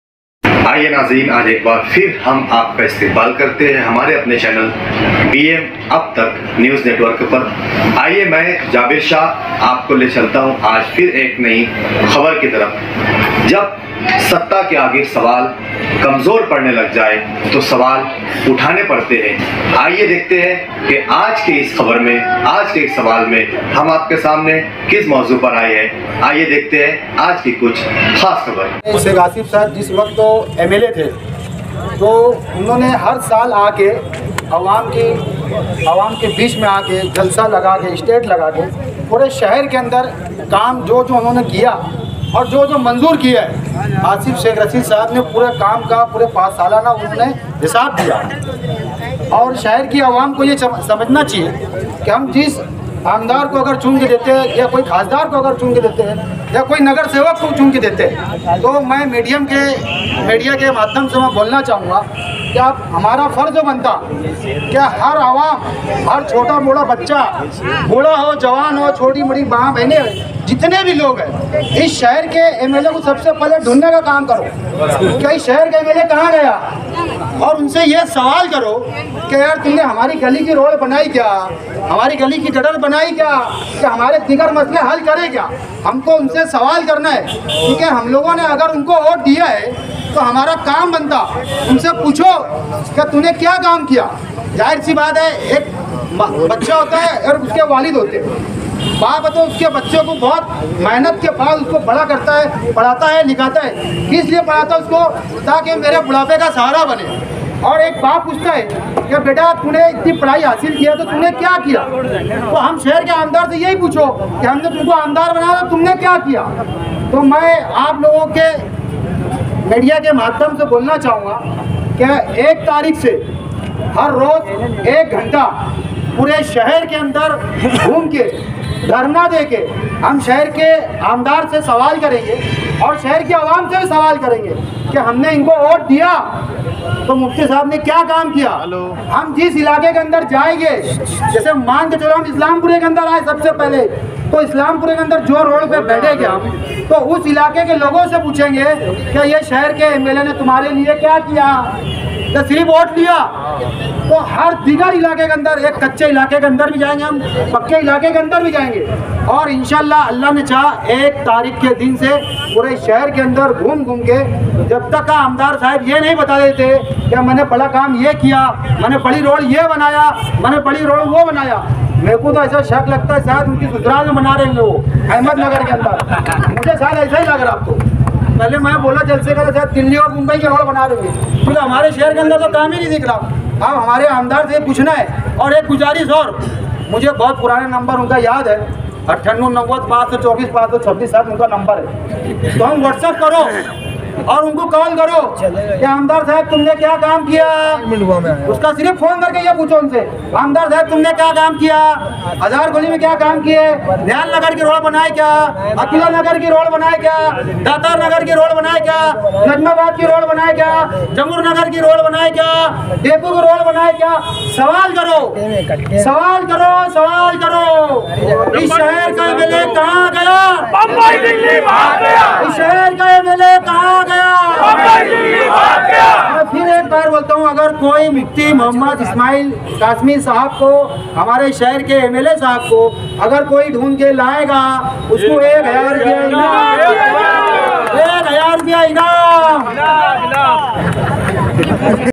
مفتی محمد اسماعیل قاسمی صاحب کو ڈھونڈ کر لانے والے کو ایک ہزار روپے کا انعام دیا جائے گا خالد حاجی
شہر کے موجودہ MLA مفتی محمد اسماعیل قاسمی صاحب کو ڈھونڈ کر لانے والوں کو ایک ہزار روپے کا انعام دیا جائے گا خالد حاجی